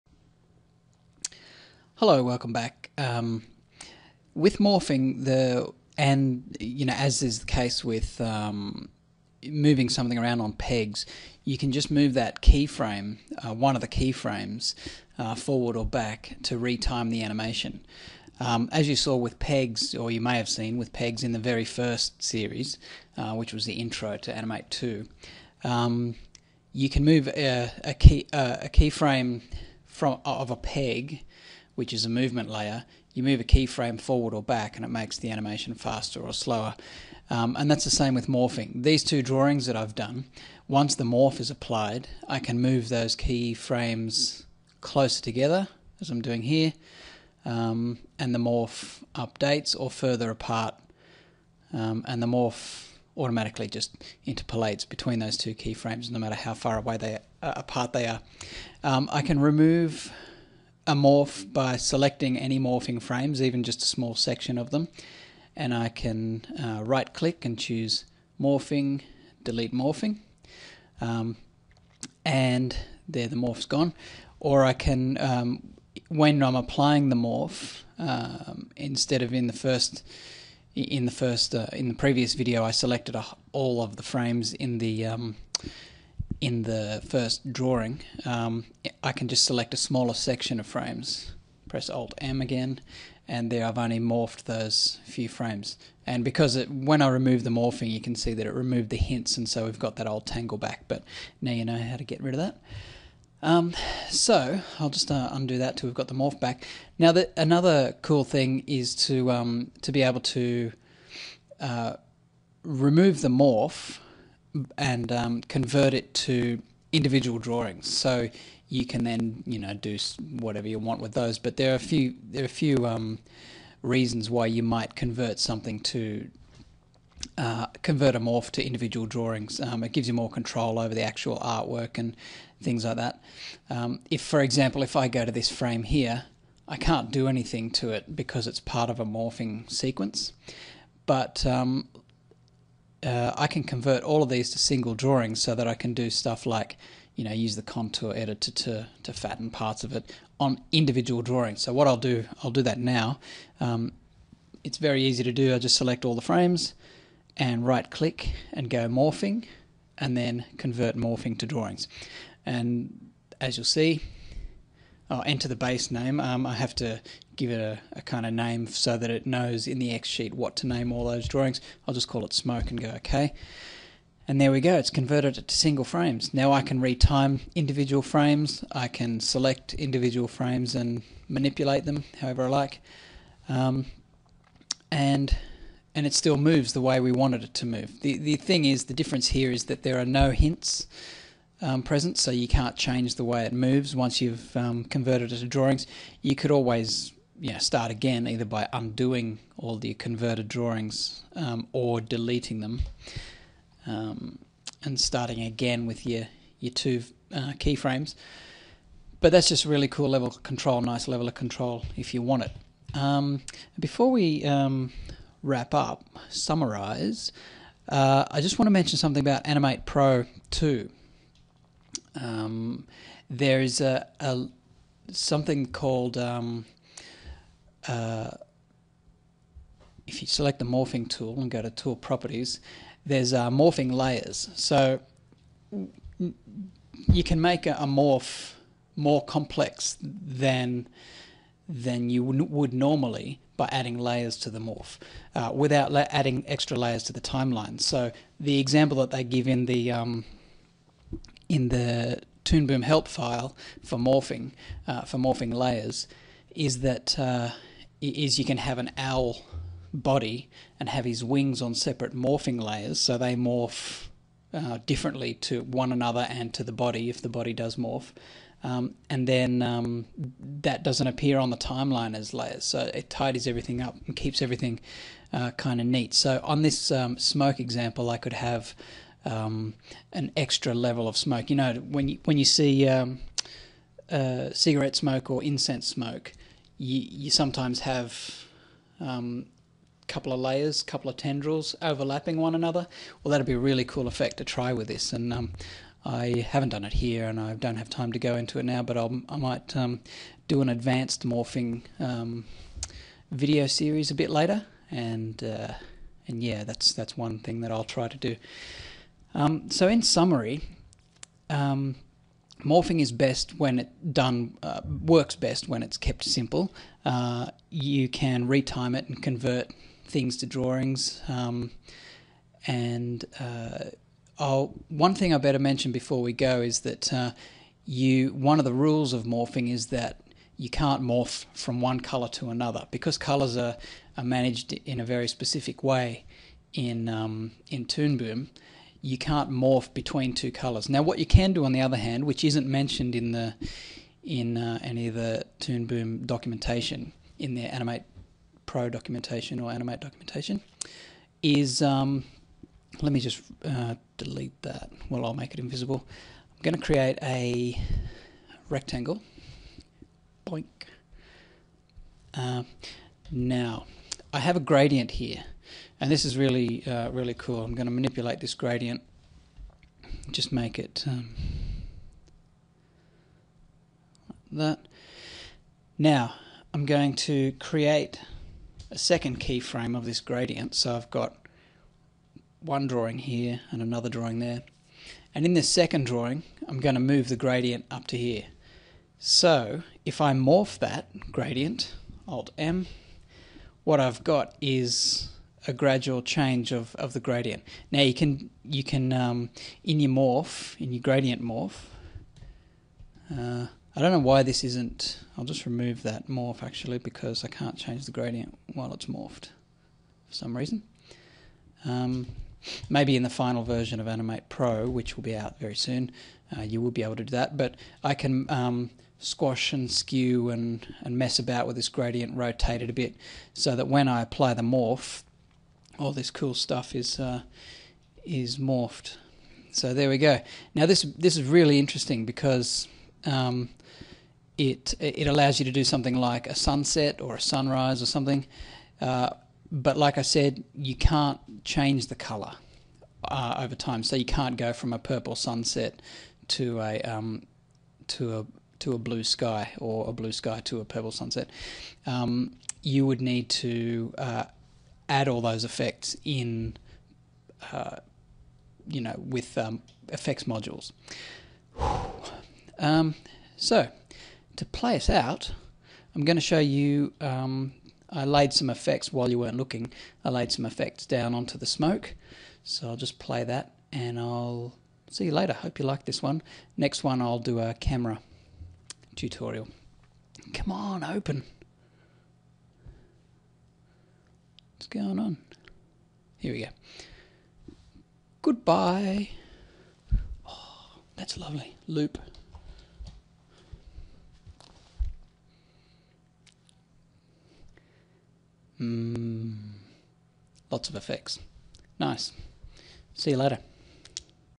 ToonBoom Animate 2 - morphing 3/3
A few closing points on converting a morph to individual drawings and retiming a morphing sequence.